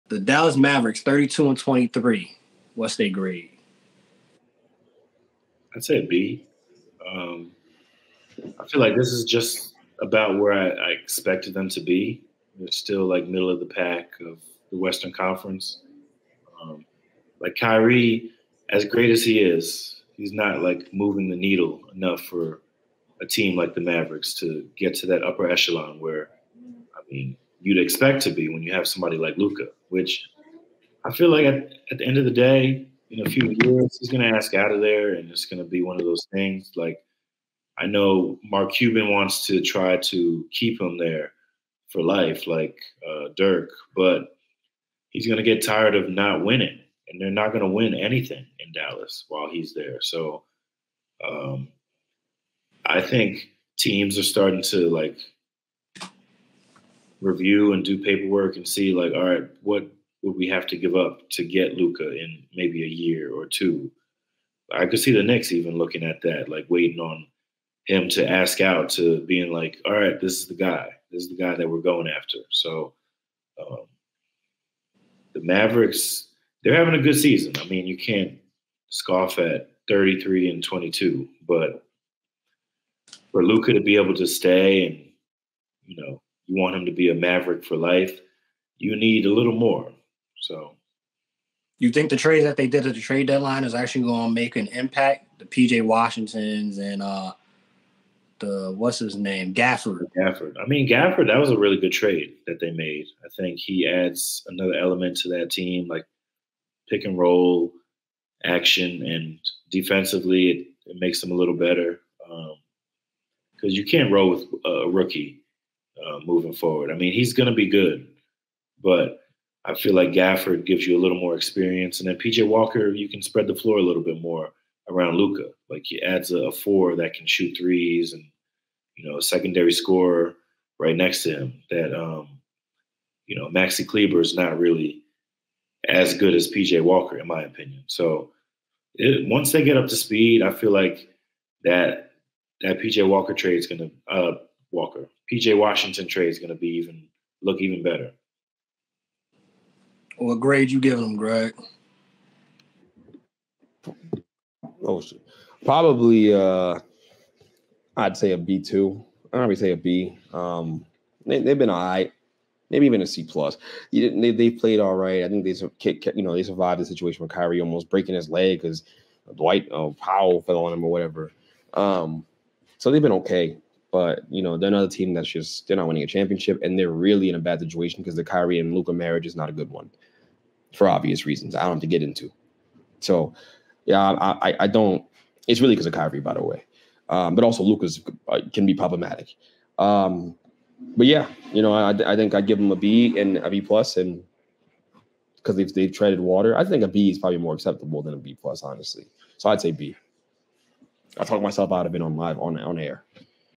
Is Kyrie and Luka Enough In The West?
Can Kyrie and Luka with the additions at the trade deadline get it done in the West?